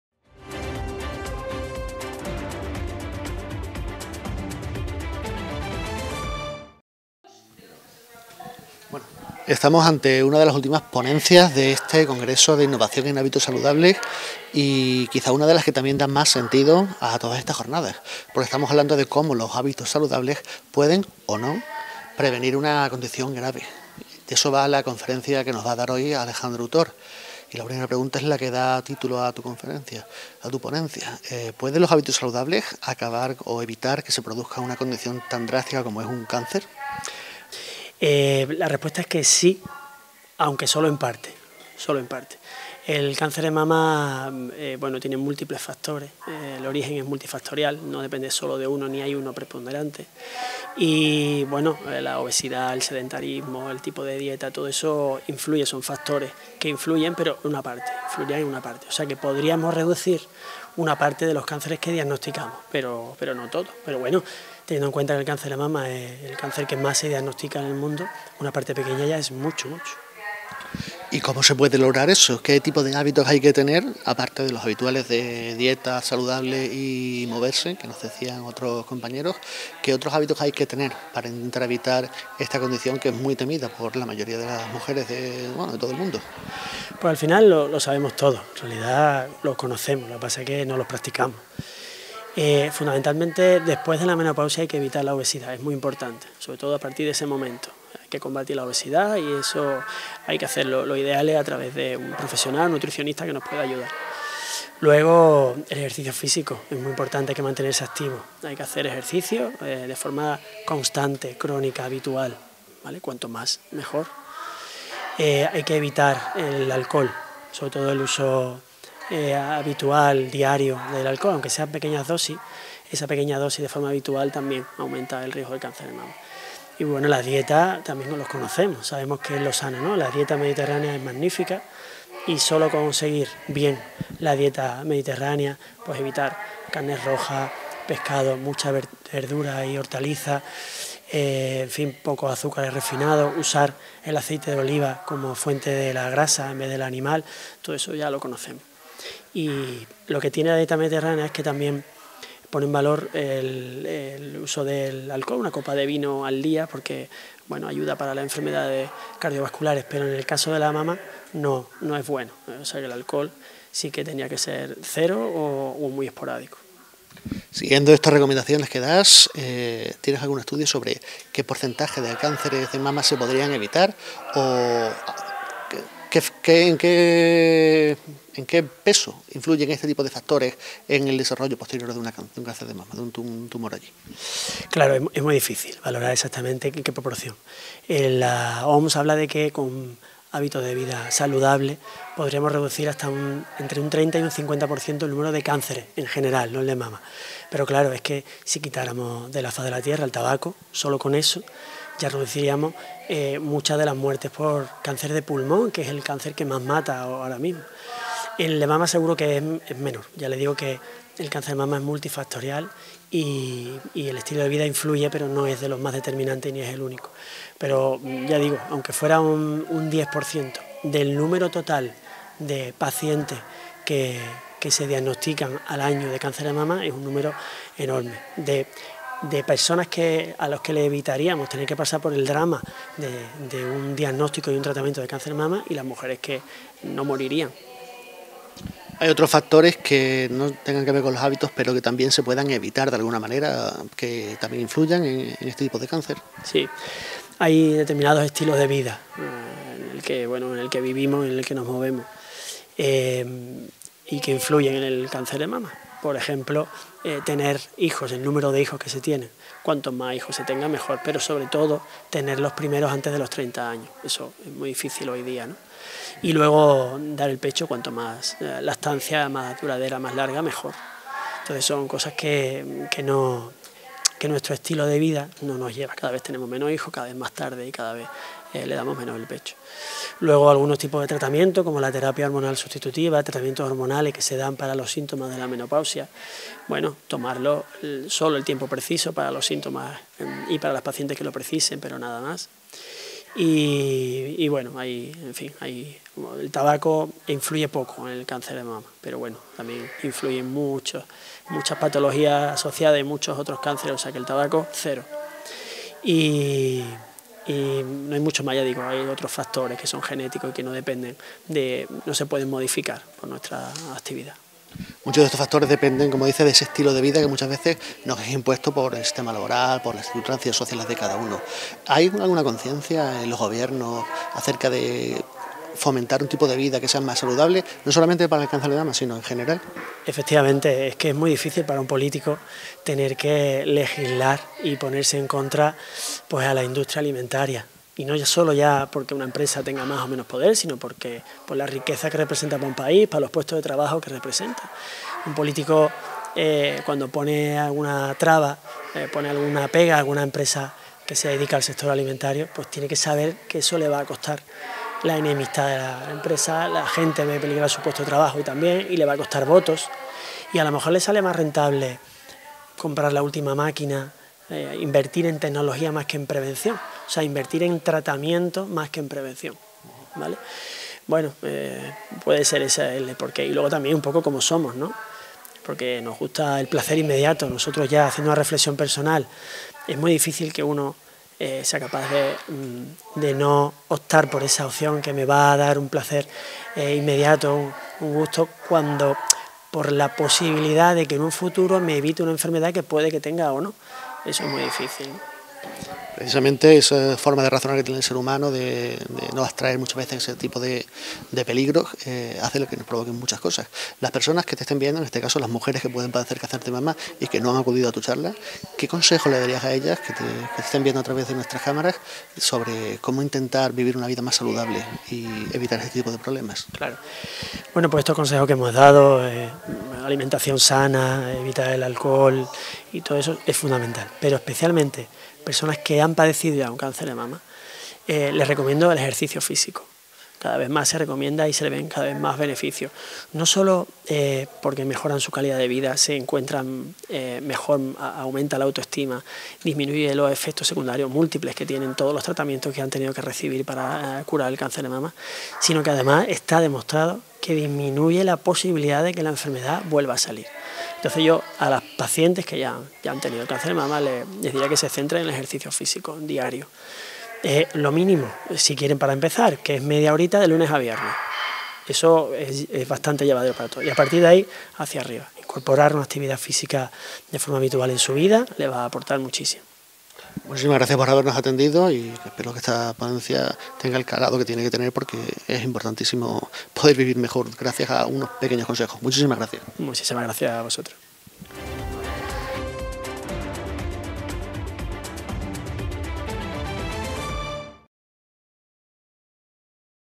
¿Pueden los hábitos saludables prevenir el cáncer de mama?
El I Congreso internacional multidisciplinar de Hábitos Saludables concluyó durante la tarde de este sábado y lo hizo con una de las conferencias más importantes, qué da sentido a todo el congreso. Una conferencia a cargo de Diego Alejandro Utor, que planteaba si mantener unos hábitos saludables permite evitar el cáncer de mama. La respuesta, a continuación.